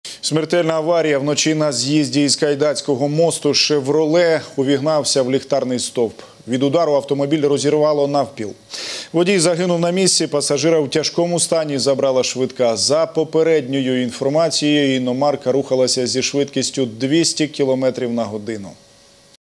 В Днепре водитель Chevrolet на скорости снес столб на Кайдакском мосту
Ночью 23 сентября в Днепре на Кайдакском мосту случилось смертельное ДТП.